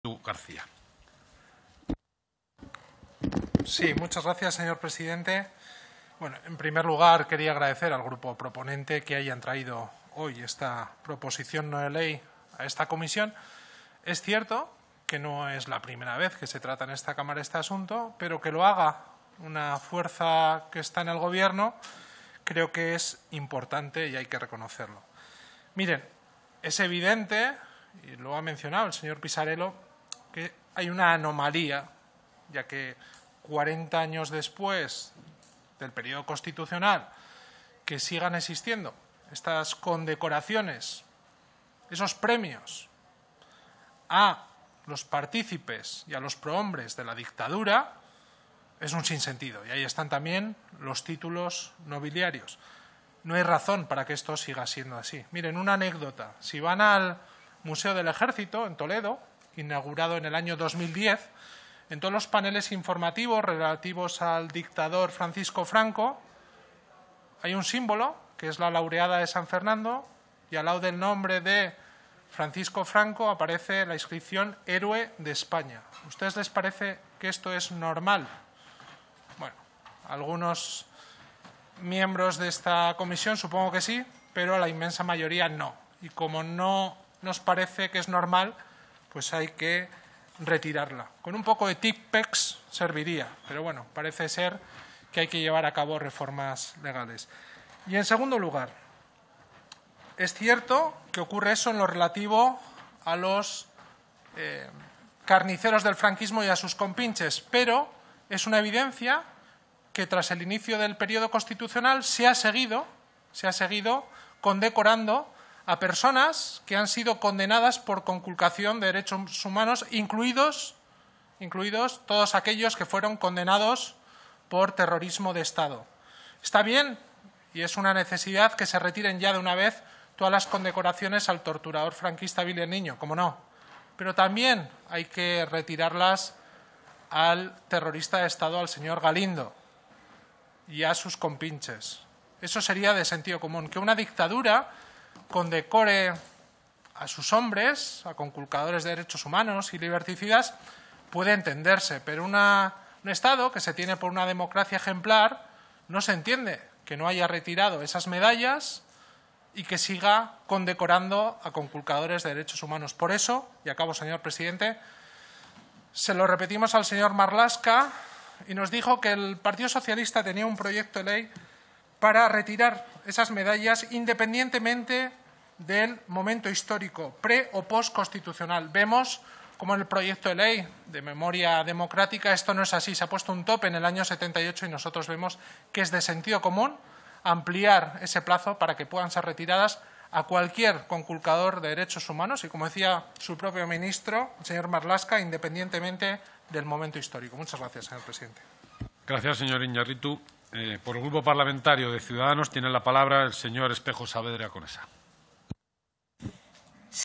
Jon Iñarritu: «Es una anomalía que tras 40 años sigan existiendo condecoraciones franquistas»
El diputado de EH Bildu, Jon Iñarritu, pide la retirada de todas las condecoraciones; las otorgadas durante el franquismo y después de la dictadura. «Es una anomalía que tras 40 años sigan existiendo condecoraciones a los prohombres de la dictadura franquista», asegura, y añade: «Después del franquismo también se siguió premiando a condenados por vulnerar derechos humanos, incluidos los condenados por el terrorismo de Estado del GAL».